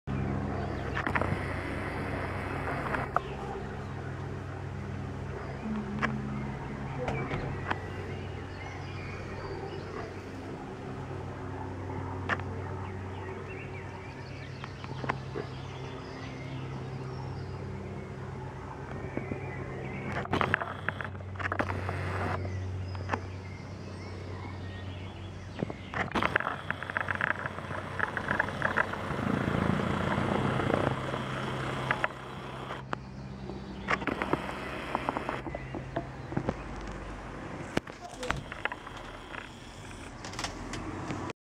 13.6.2013 17:30 Dual Plane Formation loud lowpass ständige Tiefflüge Voerde Fluglärm Schwarze Heide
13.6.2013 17:30 Dual Plane Formation loud lowpass ständige Tiefflüge Voerde Fluglärm Schwarze Heide